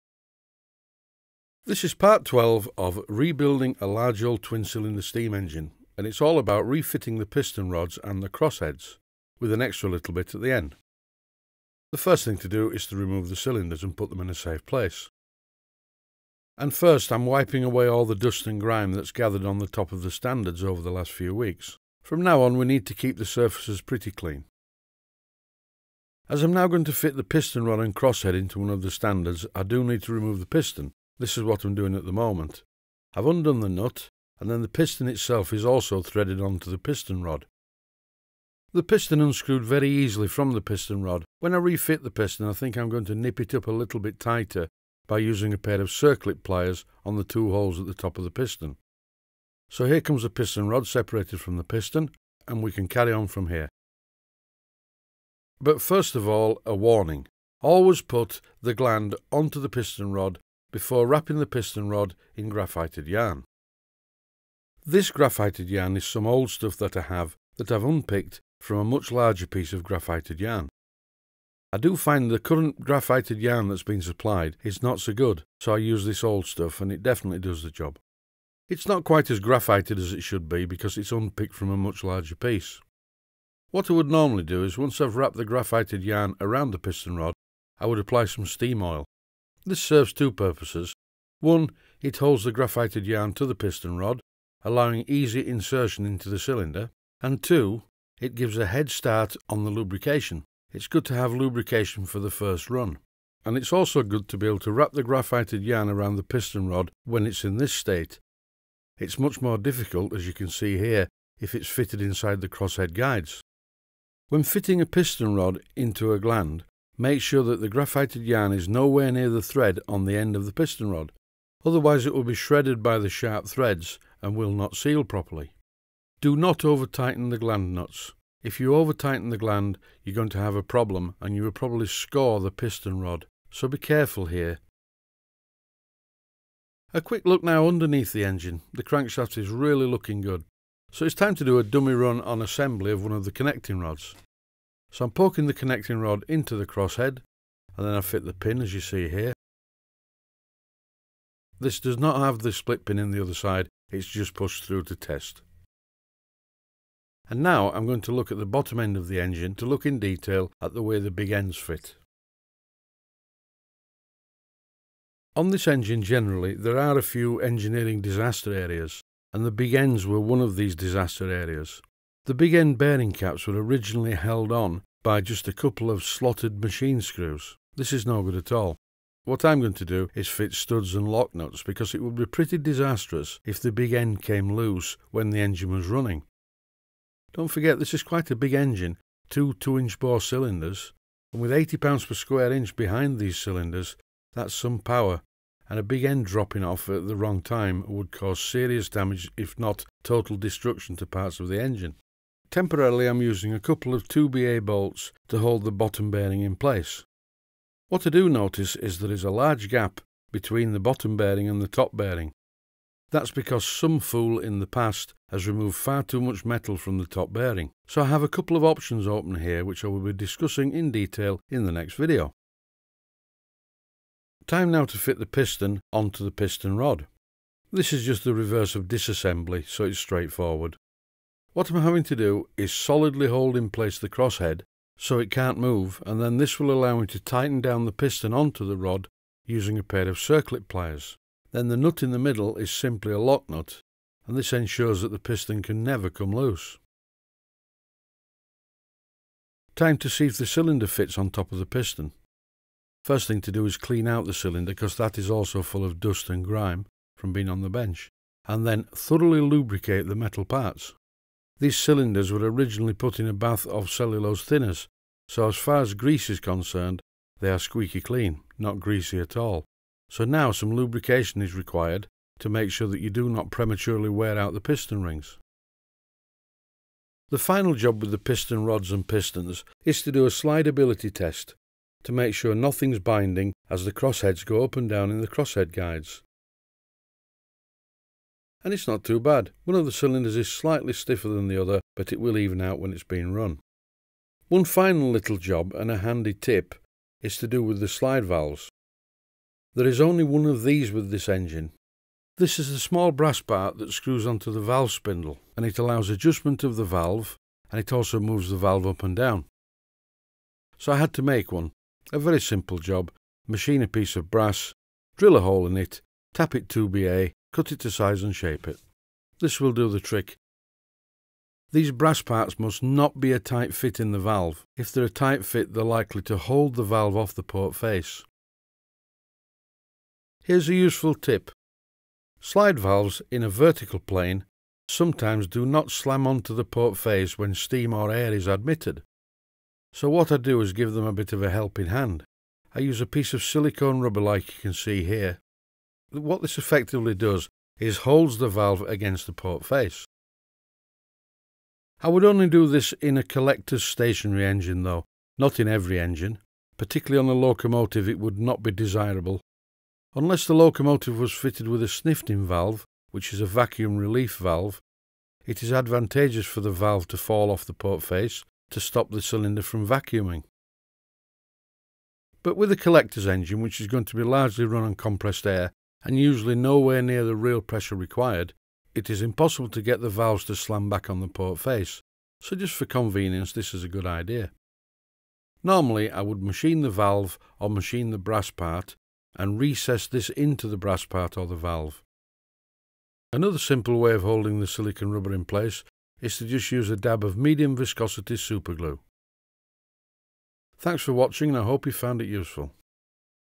PART #12 - REBUILDING A LARGE OLD TWIN CYLINDER MODEL STEAM ENGINE
This is Part #12 of "Rebuilding A Large Old Twin Cylinder Model Steam Engine". This video shows the fitting of the Piston, Rods & Crossheads plus a handy tip at the end of the video.